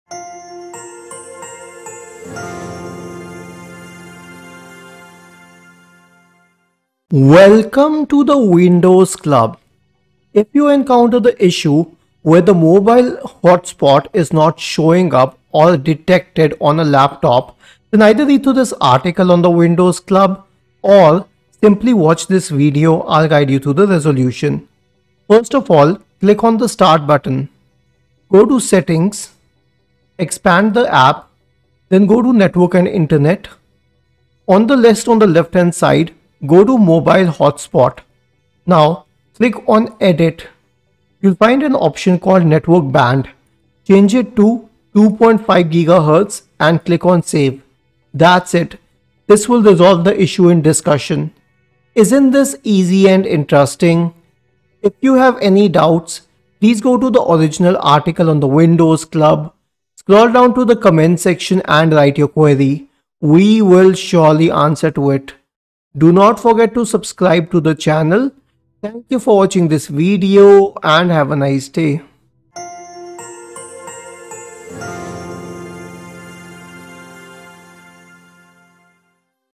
Mobile Hotspot not showing up or detected on laptop
The Mobile Hotspot feature is used to share the Internet connection via WiFi signals. These signals are later caught by other devices using WiFi and the Internet connection is shared. But some users have been reporting that the WiFi network is not visible on other devices. This could be due to various factors, but the main one here being the frequency at what the WiFi network is being broadcasted.

Timecodes:
0:00 Intro
0:24 Click on the Start button - Settings - Network & Internet
0:38 Click on Edit
0:43 Change Network Band to 2.4Ghz